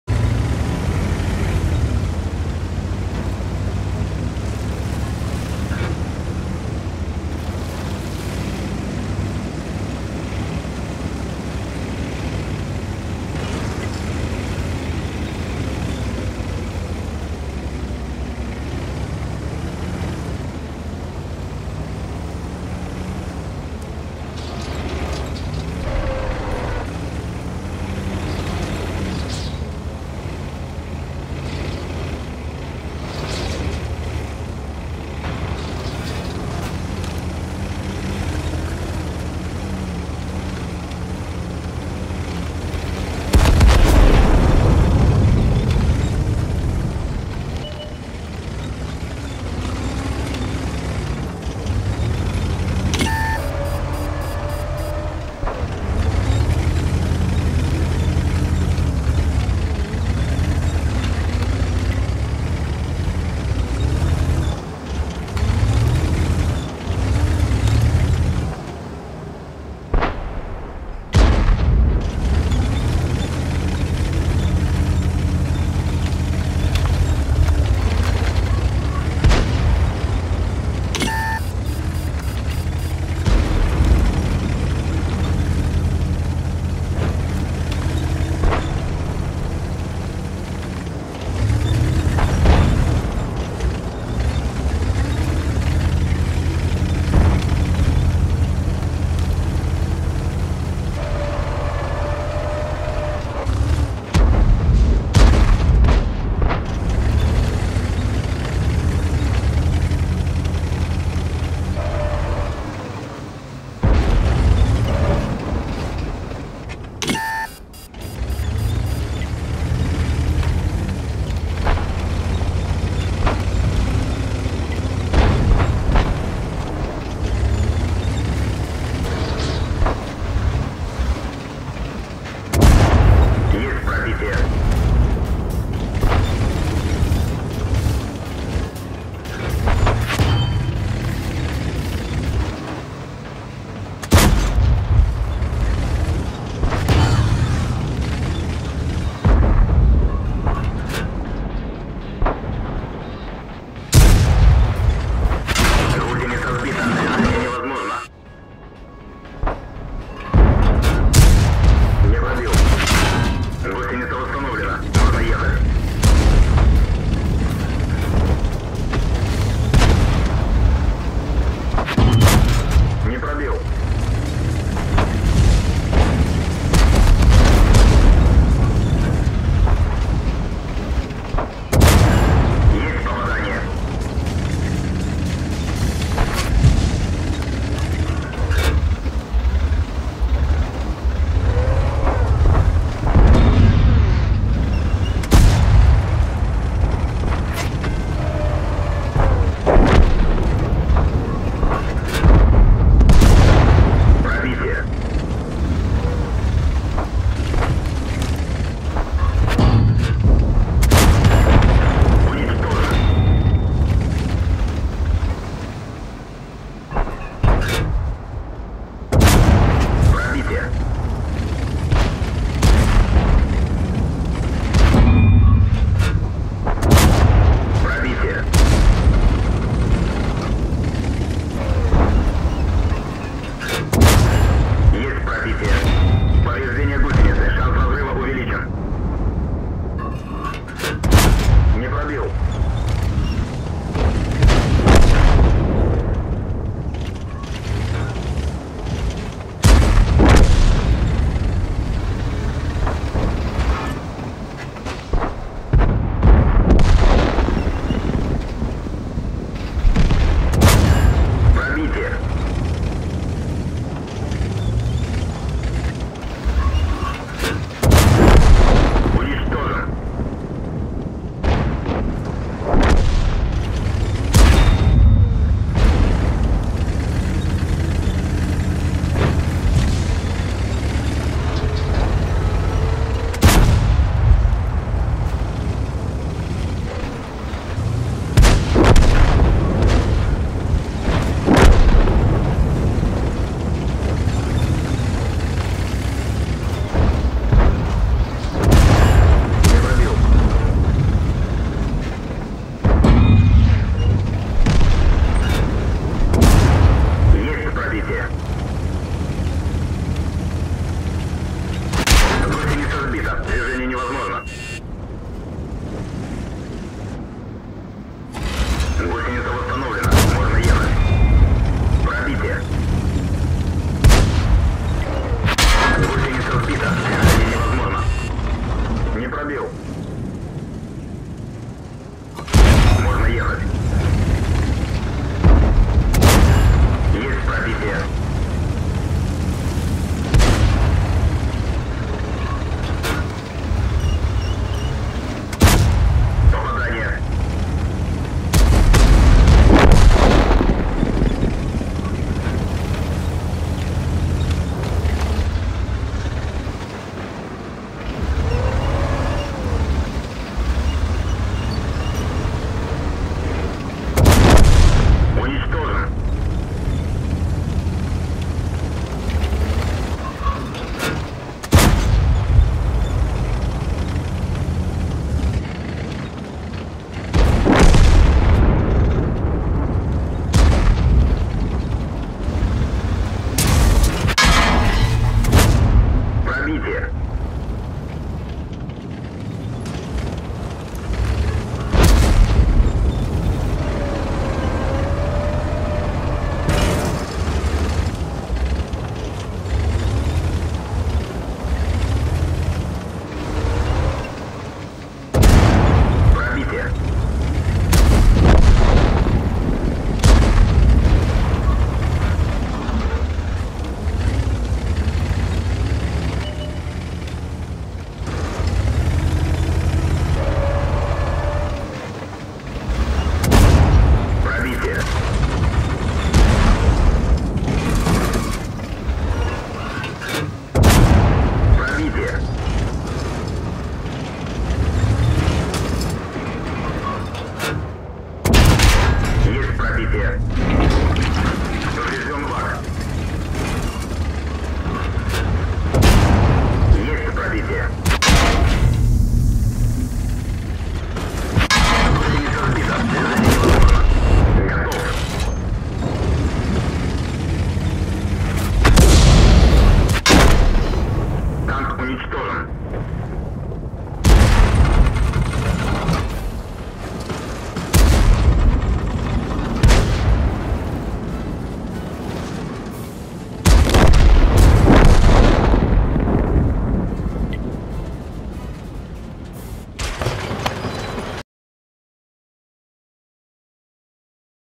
КВ-5 на ББ. 5700 урона, 5300 заблокированного уро
Игрок: Privet_iz_Kazani [CH3SS]
КВ-5 на ББ. 5700 урона, 5300 заблокированного урона. Бой с 9 лвлами.